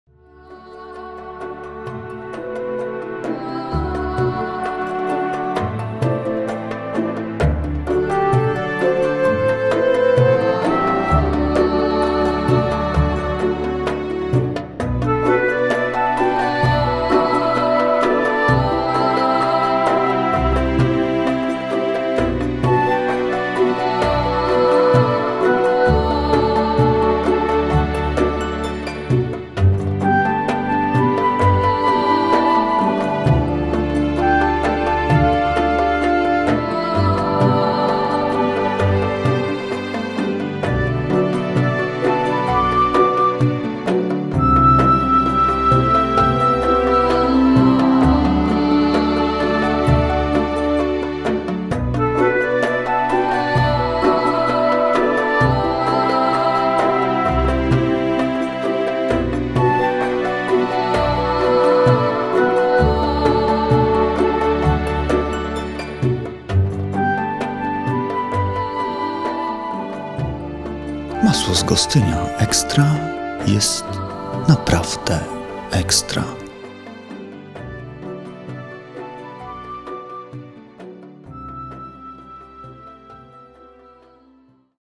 Masło Gostyńskie Ekstra
Film jest animowaną odpowiedzią na semestralne zadanie nowego designu dla dobrze znanego produktu. Padło na Masło z Gostynia. Masło jest produktem, którego nie trzeba chyba reklamować i zachwalać, co najwyżej wyróżniać. Dizajn jest klasyczny, stylizowany na produkt z długą historią. Hasło reklamowe wręcz oczywiste. Produkcja ze stycznia 2011